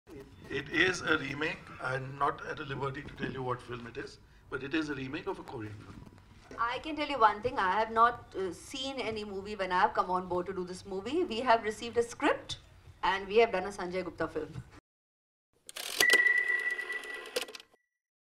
Jazbaa Trailer Launch: Is Sanjay Gupta's Film a Remake?
Even before Jazbaa's trailer had been released, there were reports suggesting that the film is a remake of a Korean film. Director Sanjay Gupta and Aishwarya Rai finally clear the air.